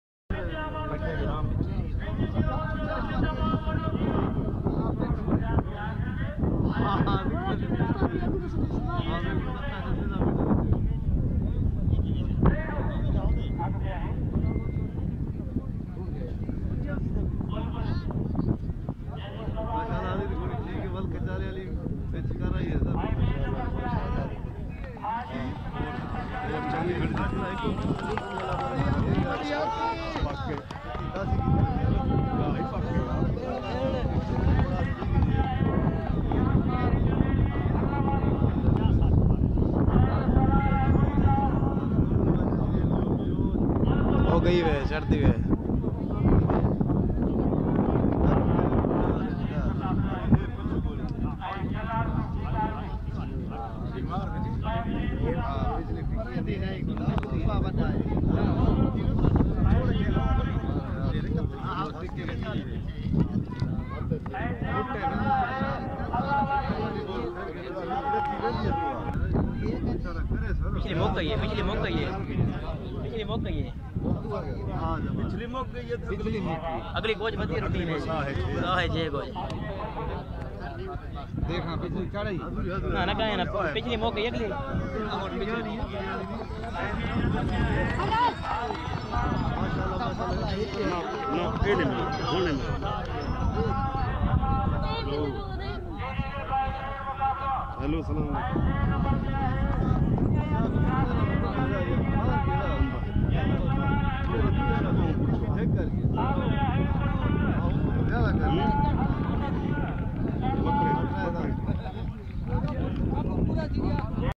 Bulbul.. Jalal Pur Singhar.... Allah Wali....... Bulbul win Sardar Riaz Mehmood Khan Mazarii.
All Pakistan pacer champion.. Mare Bulbul defeated Jalal pur singhar and Allah Wali...All Pakistan Horse  Race Sadiq Abad..